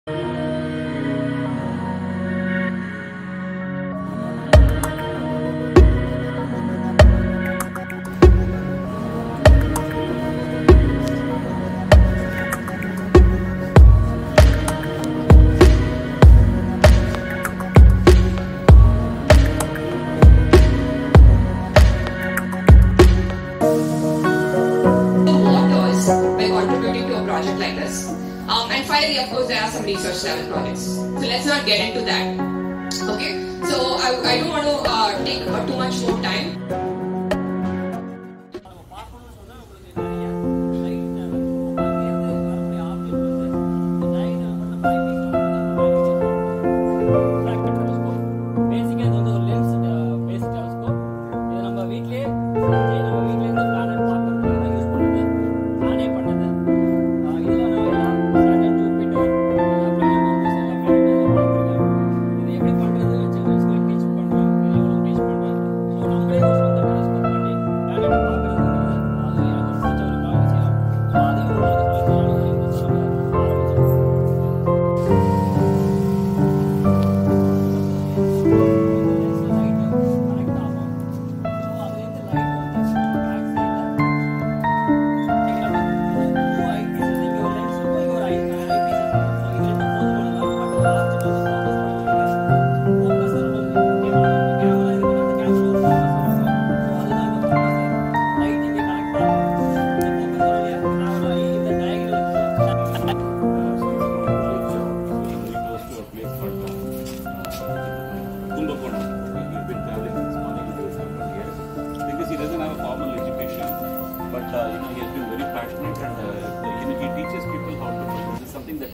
Astronomy Workshop in srm university Chennai
Frist time I Collab with Naxxatra science
I happy join us team
Astronomy Workshop in Collab with Naxxatra science at SRM university
Naxxatra science